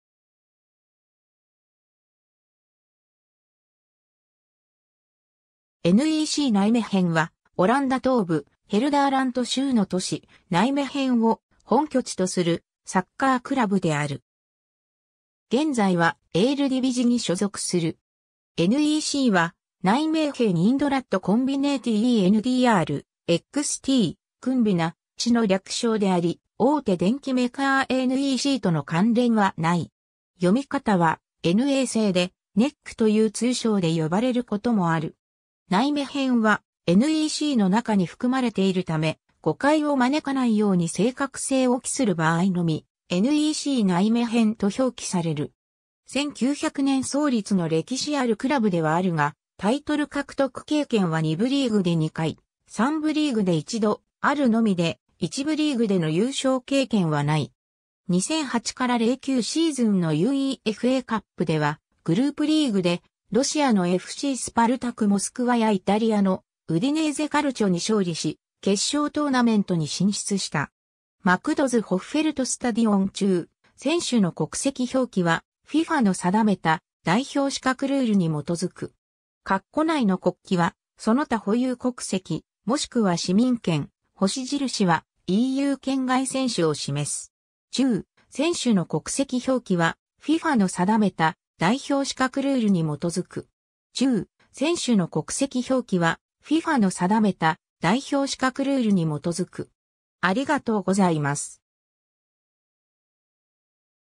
NECナイメヘン
NECナイメヘン（NEC Nijmegen、通称NEC（N.E.C. (発音 [ˌɛneːˈseː])）は、オランダ東部、ヘルダーラント州の都市ナイメヘンを本拠地とするサッカークラブである。
現在はエールディヴィジに所属する。
 NECは「Nijmegen Eendracht Combinatie (オランダ語発音: [ˈnɛimeːɣə(n) ˈeːndrɑxt ˌkɔmbiˈnaː(t)si])」の略称であり、大手電器メーカーNECとの関連はない。
読み方は「エヌ・エー・セー」で、「ネック」という通称で呼ばれることもある。
「ナイメヘン」はNECの中に含まれているため、誤解を招かないように正確性を期する場合のみ「NECナイメヘン」と表記される。
 1900年創立の歴史あるクラブではあるが、タイトル獲得経験は2部リーグ（エールステ・ディヴィジ）で2回、3部リーグ（トゥヴェーデ・ディヴィジ）で1度あるのみで、1部リーグ（エールディヴィジ）での優勝経験はない。
 2008-09シーズンのUEFAカップではグループリーグでロシアのFCスパルタク・モスクワやイタリアのウディネーゼ・カルチョに勝利し、決勝トーナメントに進出した。
 McDOSホッフェルトスタディオン 注：選手の国籍表記はFIFAの定めた代表資格ルールに基づく。
 ※括弧内の国旗はその他保有国籍、もしくは市民権、星印はEU圏外選手を示す。